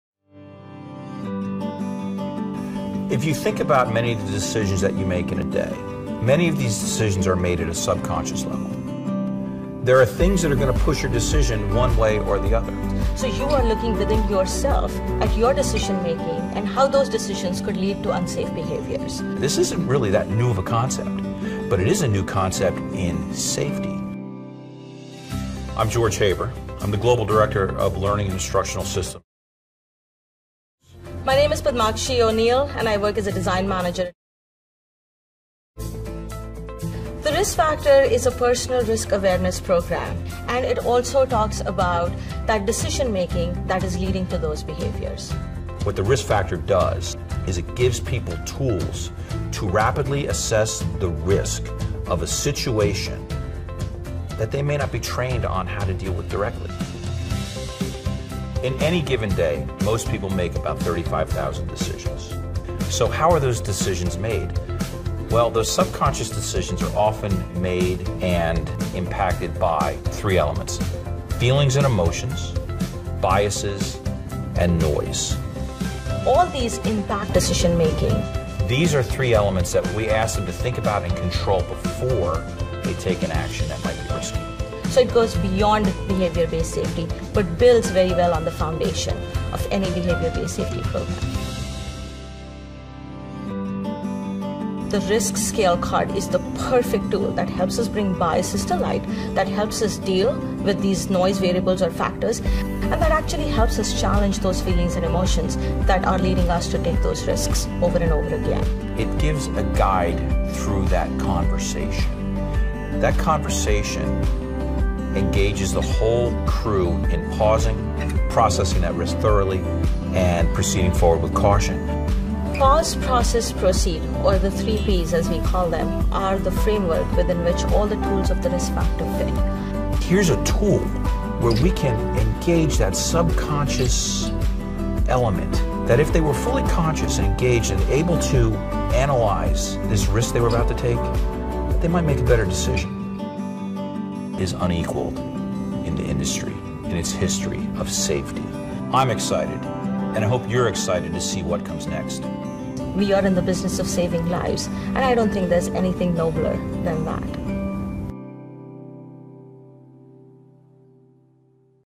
dss⁺ Risk Factor™
Why do people take unnecessary risks, even if they already know safety rules and regulations? The answer lies in neuroscience, whose insights comprise the foundation of our training, dss⁺ Risk Factor™.

Dr. George Haber explains how an understanding of motivations, emotions, and decision-making can help us reduce risk-taking, encourage safe behaviour, and transform safety cultures. Watch the interview and learn more about The Risk Factor.